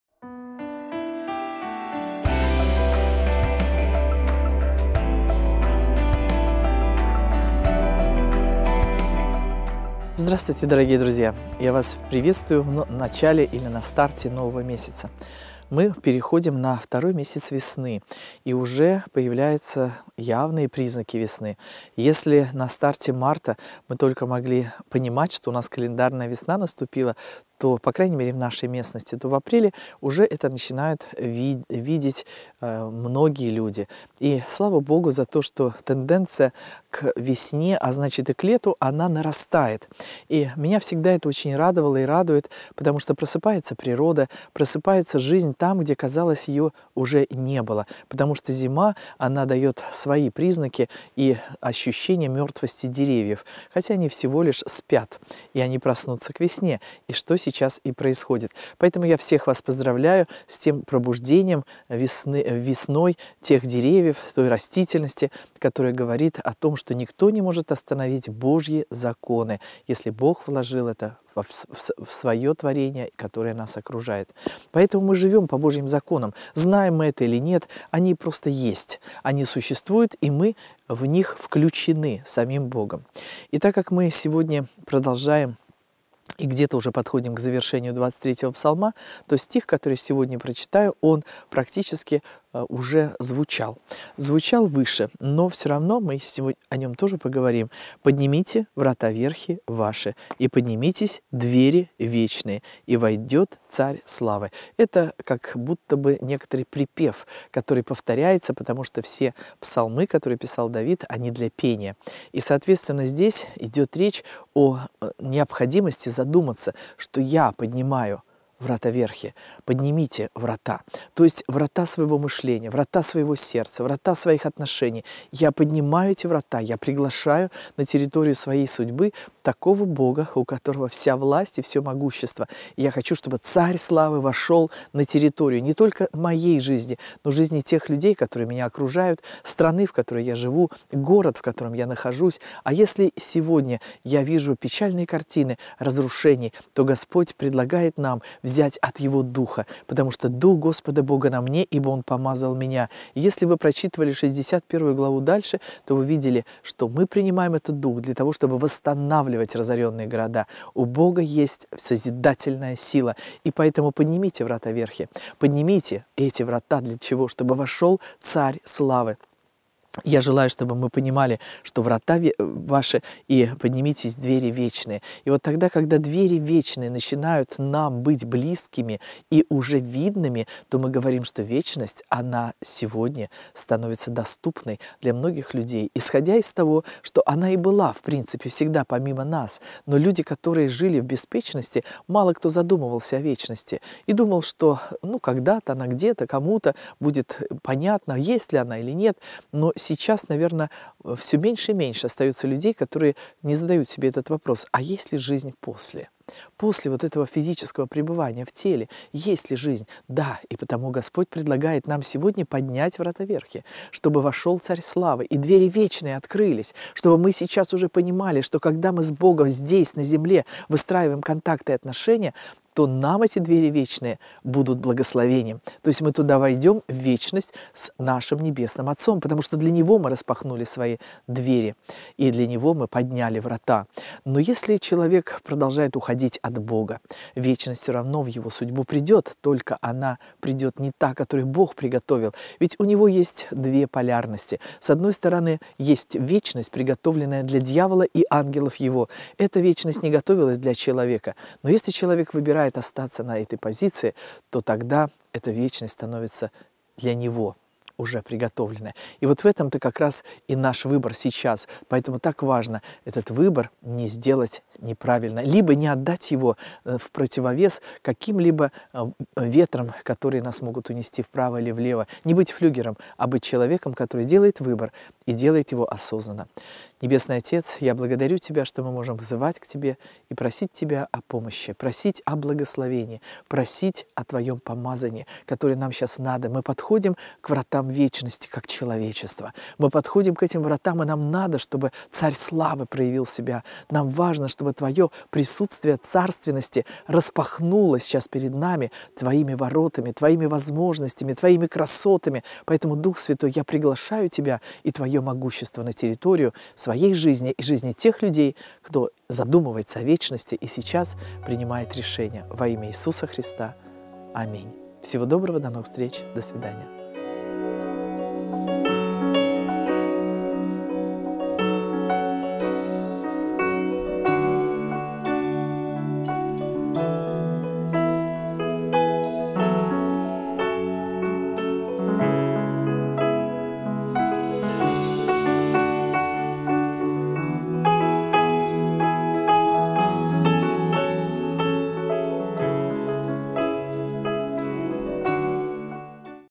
2378. Хлеб на каждый день. Псалом 23:9
2378. Хлеб на каждый день. Псалом 23:9

"9. Поднимите, врата, верхи ваши, и поднимитесь, двери вечные, и войдет Царь славы!"
(Псалтирь 23:9)